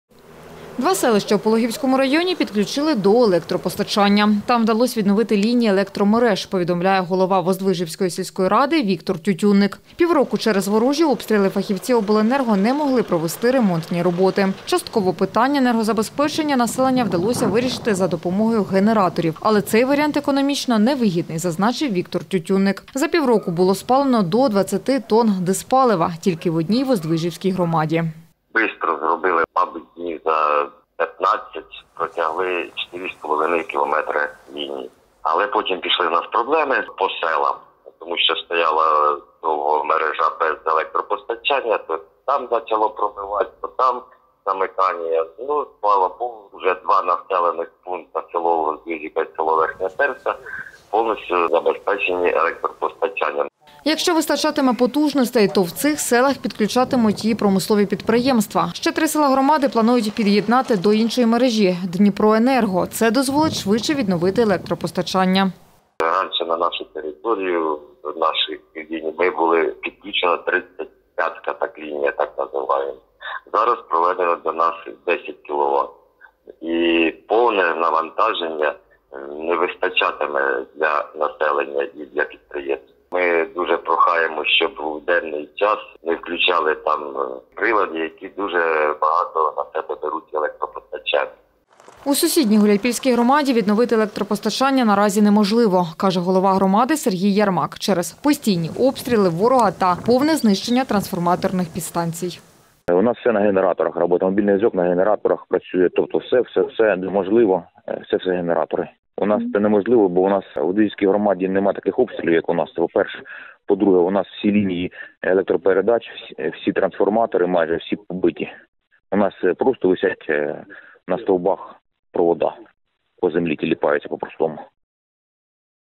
У Пологівському районі двом селищам відновили електропостачання | Новини
Два селища у Пологівському районі підключили до електропостачання. Там вдалось відновити лінії електромереж - повідомляє голова Воздвижівської сільської ради Віктор Тютюнник. Пів року через ворожі обстріли фахівці обленерго не могли  провести ремонтні роботи. Частково питання енергозабезпечення населення вдалося вирішити за допомогою генераторів, але цей варіант економічно невигідний, зазначив Віктор Тютюнник. За пів року було спалено до 20 тон дизпалива тільки в одній Воздвижівській громаді.